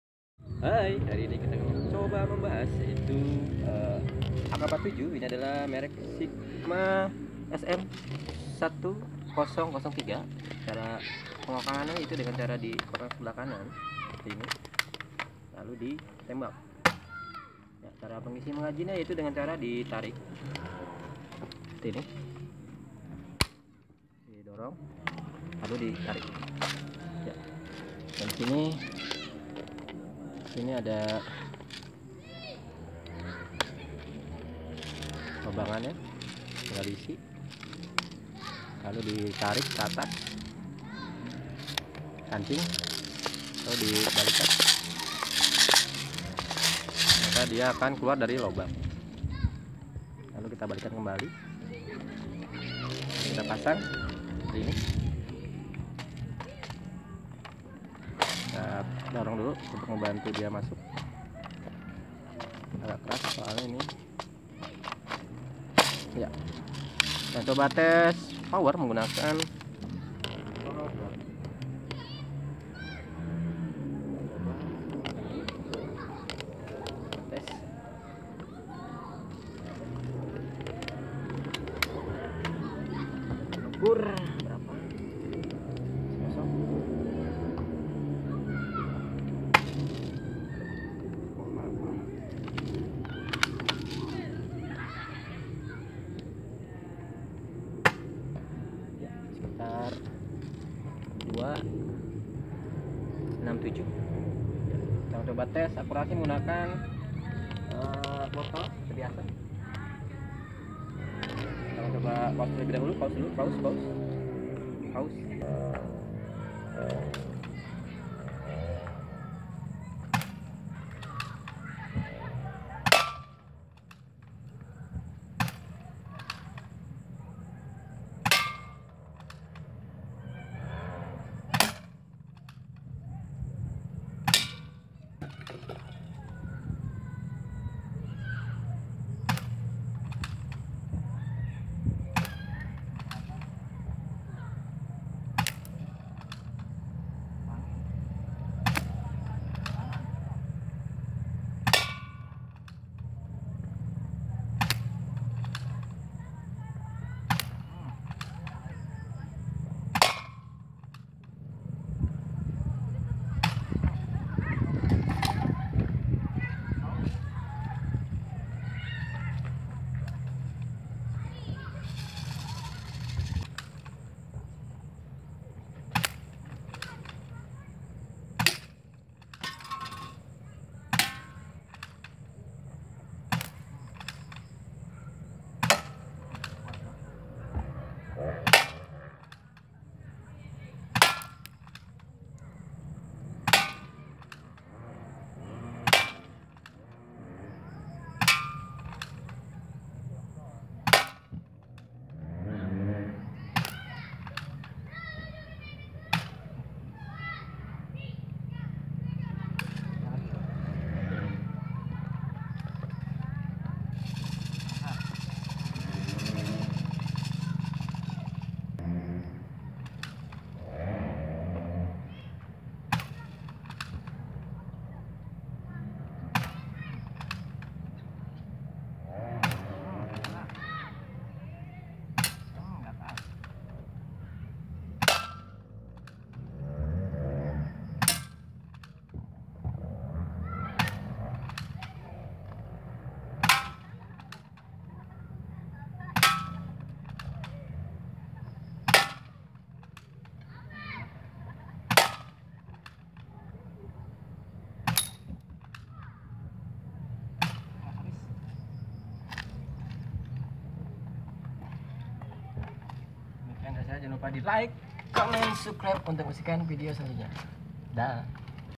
AK 47 SKYMA SM 1003C AIRSOFTGUN SPRING MADE IN CHINA
Tembakan mainan Spring AK 47 SKYMA 1003C buatan Cina menggunakan bb bullet 0,20gr (267 fps)